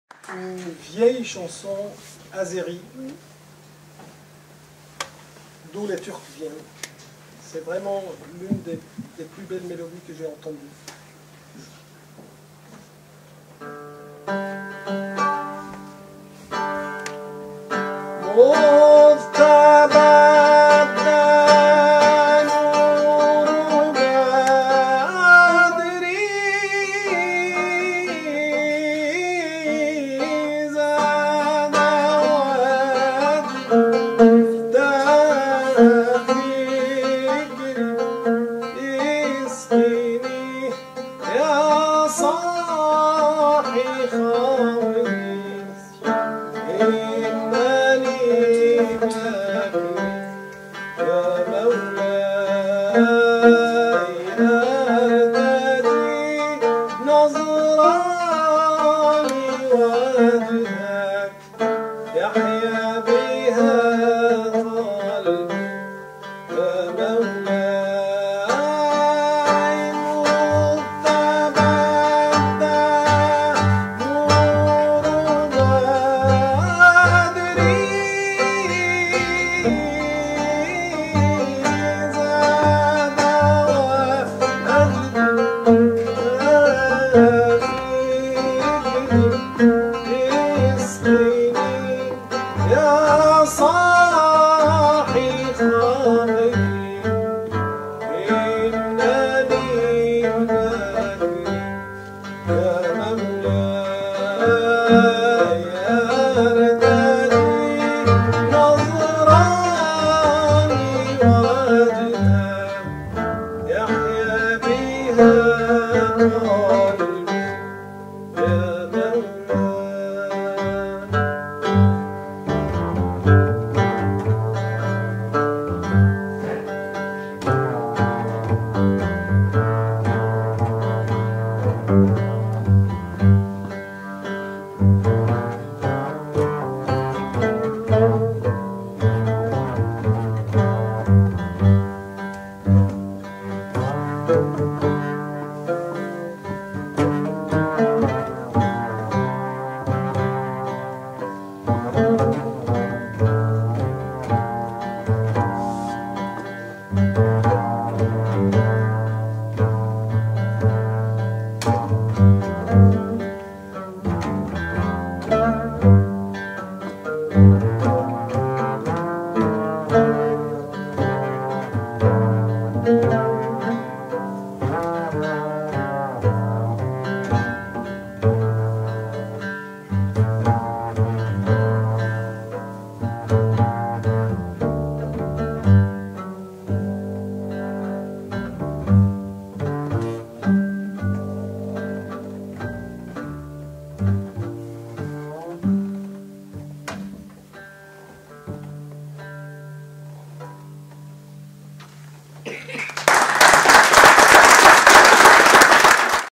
Fawaz Baker - Michel THouseau
Spectacle de Fawaz Baker (ex directeur du conservatoire d'Alep) et Michel Thouseau contrebassiste éclectique à l'espace Tivoli (Bourges) organisé par "l'Association Medina" à l'occasion de la quinzaine syrienne.